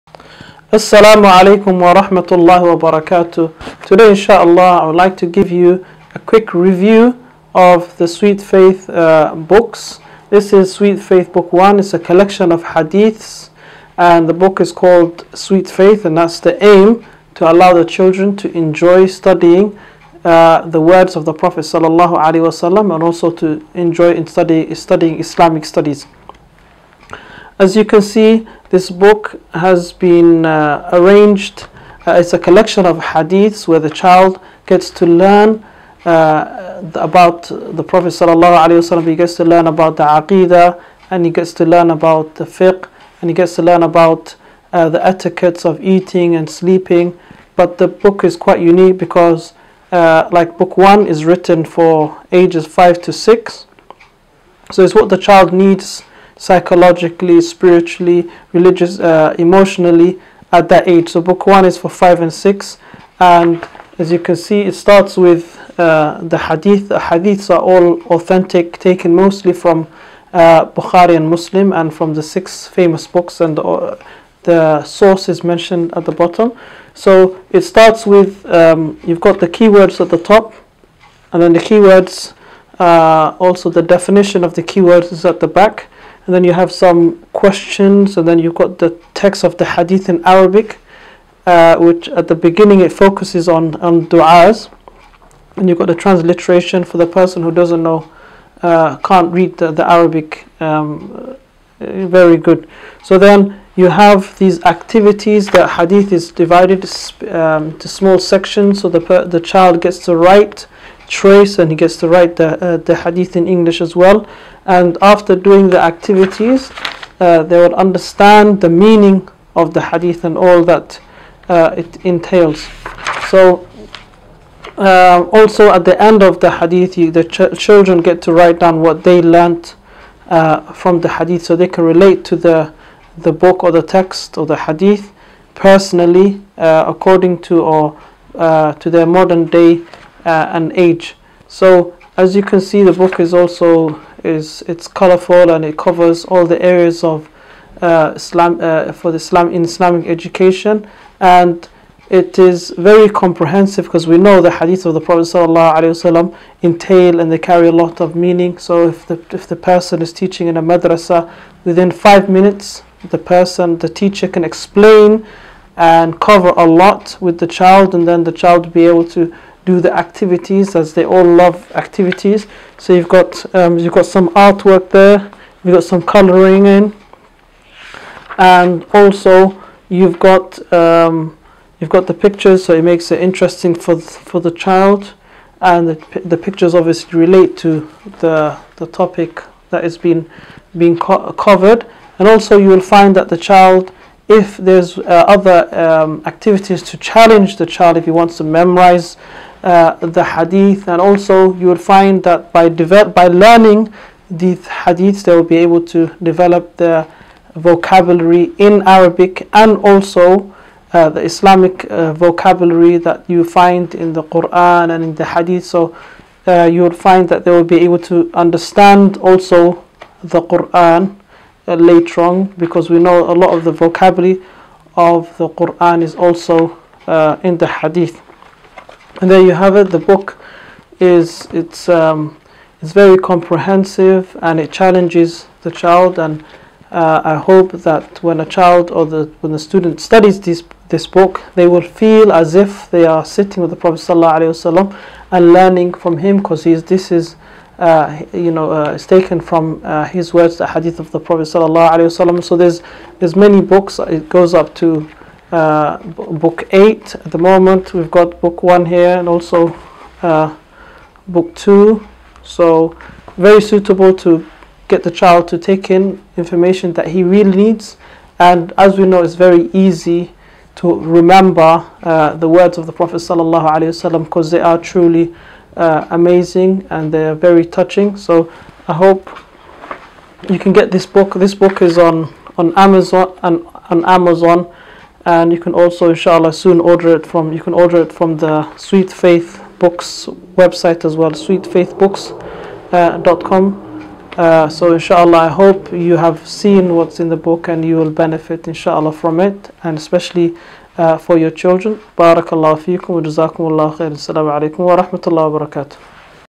Sweetfaith books a quick review.
This video is a quick review of what the Sweetfaith books are especially book 1.